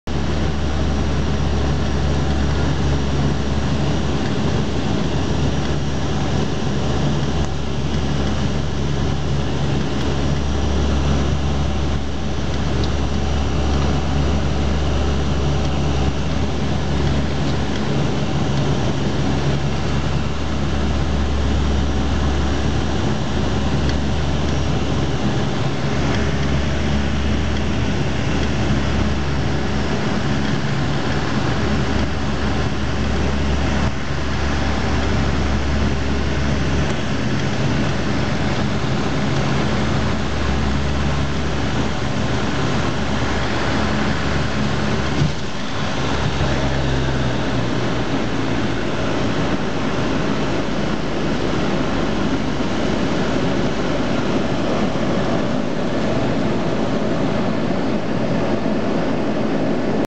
Havilland Canada Dash 8 Landing Toronto Airport 1
May-24-2007, Air Canada Flight #7826 is approaching to the Pearson International Airport...